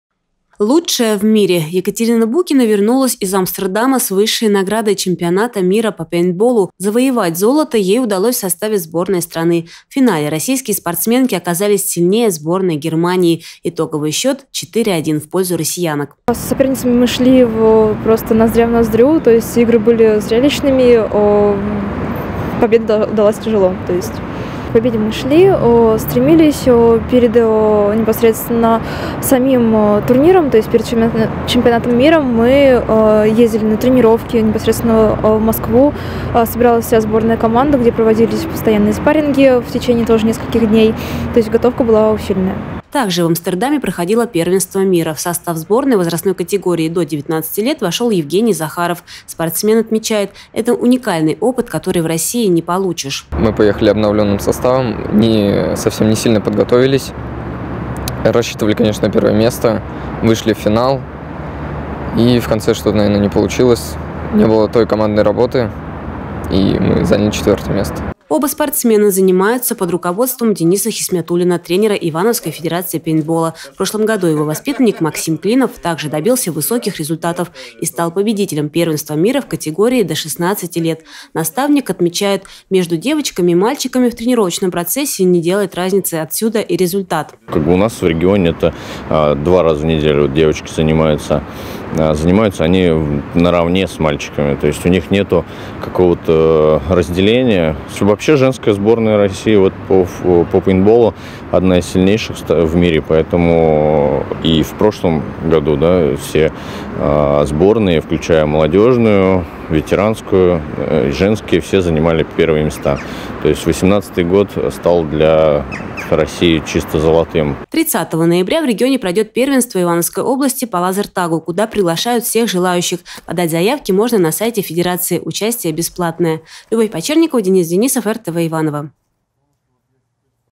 Золото из Амстердама
Лучшая в мире. Екатерина Букина вернулась из Амстердама с высшей наградой Чемпионата Мира по пейнтболу. Завоевать золото ей удалось в составе сборной страны. В финале российские спортсменки оказались сильнее сборной Германии.